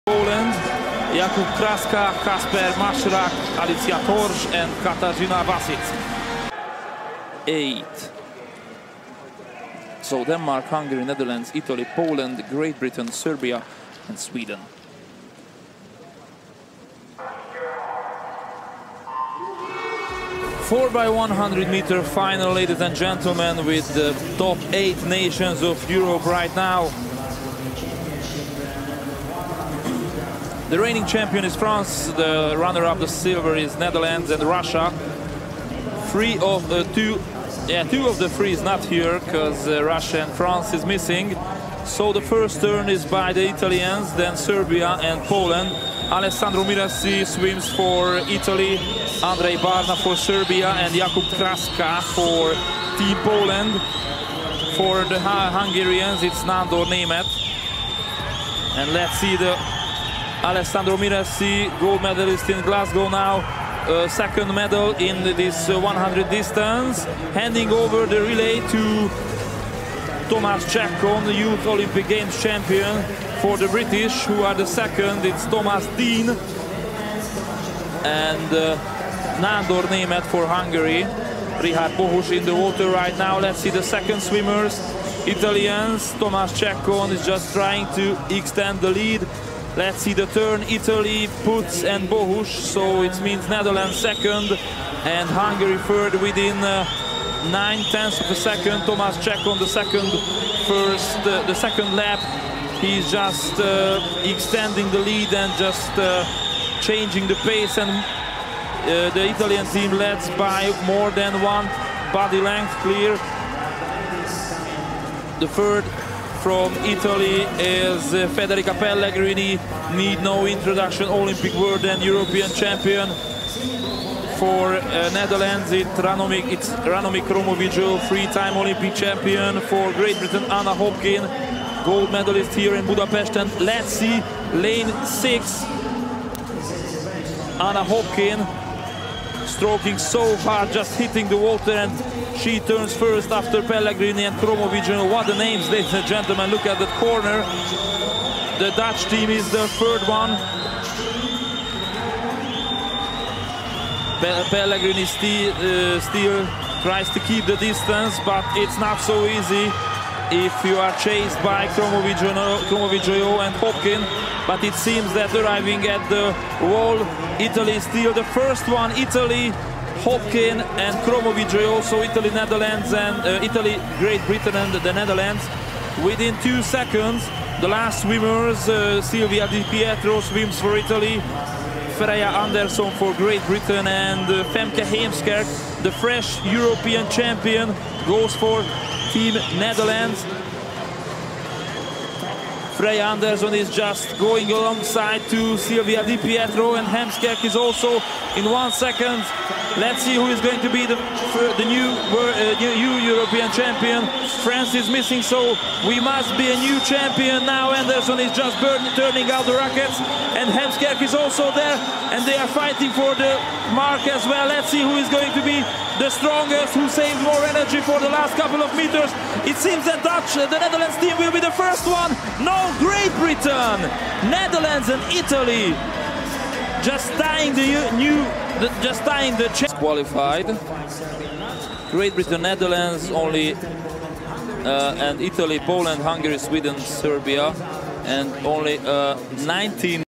3:25.59 REKORD POLSKI sztafeta 4 x 100 m dow. MIX J. Kraska, K. Majchrzak, A. Tchórz, K. Wasick
3:25.59 REKORD POLSKI 🇵🇱 sztafeta 4 x 100 m stylem dowolnym MIX

Skład: Jakub Kraska, Kacper Majchrzak, Alicja Tchórz, Katarzyna Wasick

 4.  miejsce w finale Mistrzostw Europy w Budapeszcie 2021🔥